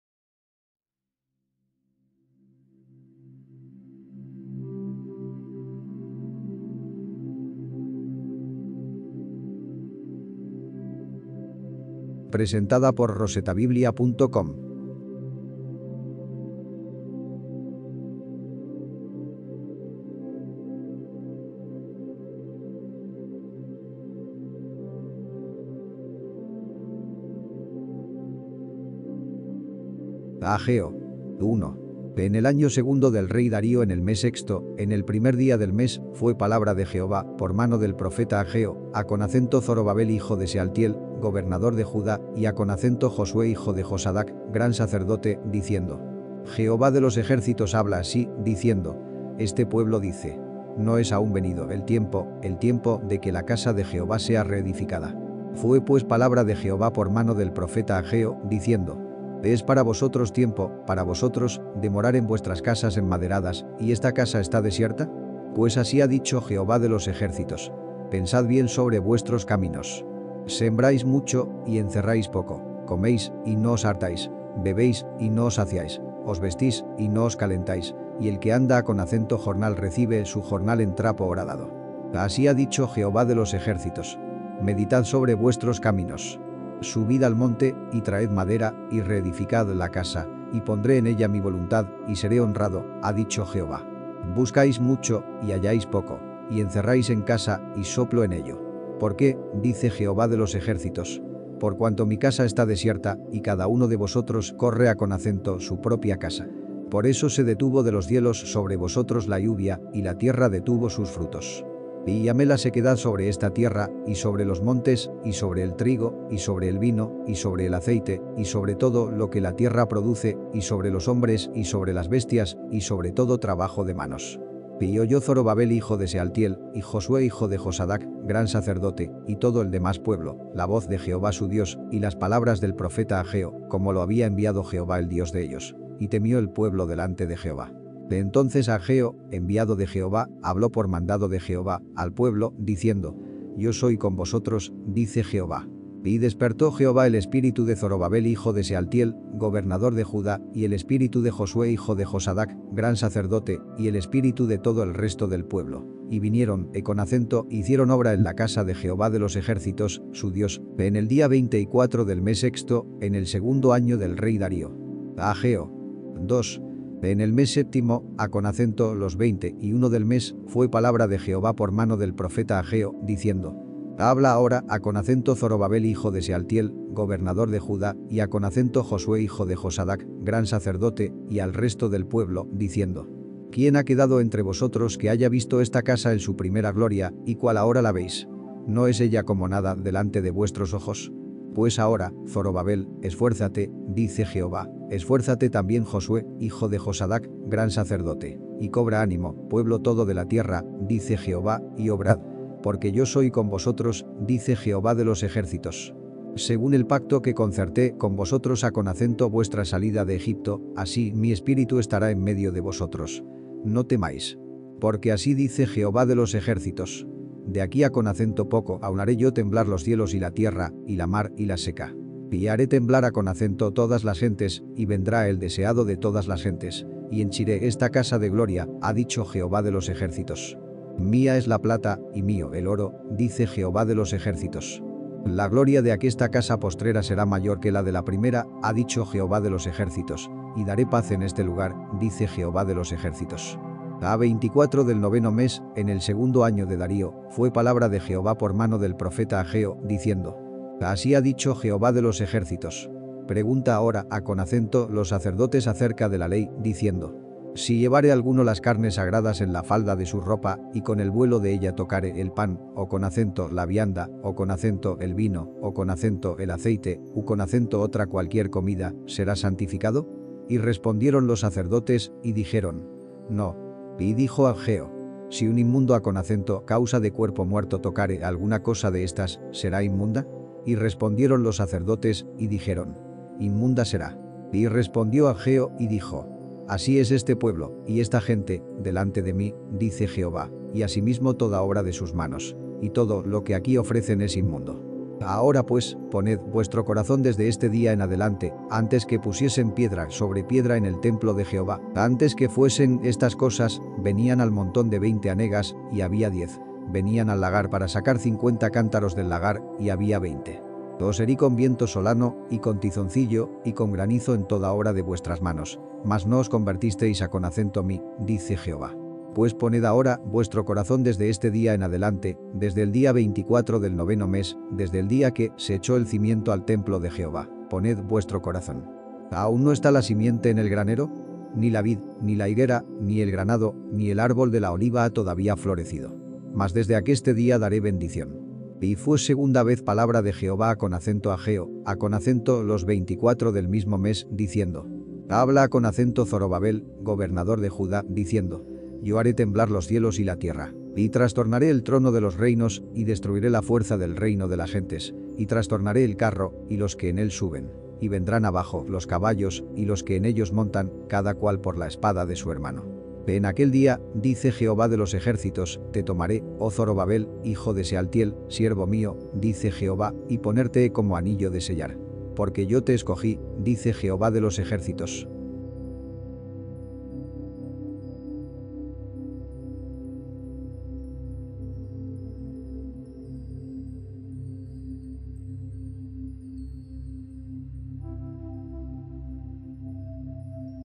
37. Hageo (El Antiguo Testamento en Español)
El libro de Hageo es parte del Antiguo Testamento de la Biblia, es una profecía escrita por el profeta Hageo, que insta a la gente de Judá a reconstruir el templo en Jerusalén, y a priorizar sus deberes religiosos sobre sus propias necesidades materiales, y les promete bendiciones y prosperidad como recompensa por su obediencia y fidelidad a Dios.